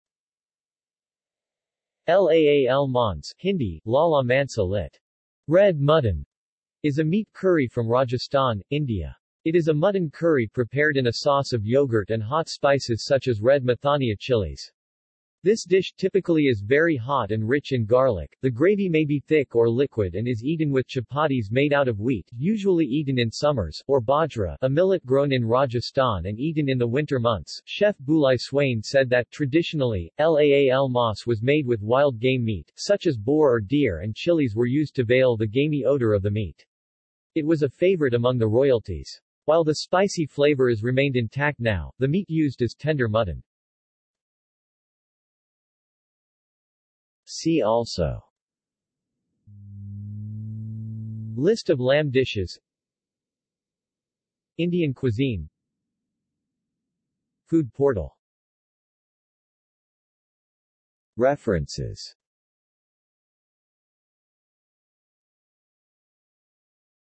Laal maans | Wikipedia audio article
This is an audio version of the Wikipedia Article:
Laal maans

SUMMARY
Laal maans (Hindi: लाल मांस; lit. "red mutton")  is a meat curry from Rajasthan, India. It is a mutton curry prepared in a sauce of yoghurt and hot spices such as red Mathania chillies. This dish typically is very hot and rich in garlic, the gravy may be thick or liquid and is eaten with chapatis made out of wheat (usually eaten in summers) or bajra (a millet grown in Rajasthan and eaten in the winter months).Chef Bulai Swain said that: "Traditionally, laal maas was made with wild game meat, such as boar or deer and chillies were used to veil the gamy odour of the meat. It was a favourite among the royalties. While the spicy flavour is remained intact now, the meat used is tender mutton."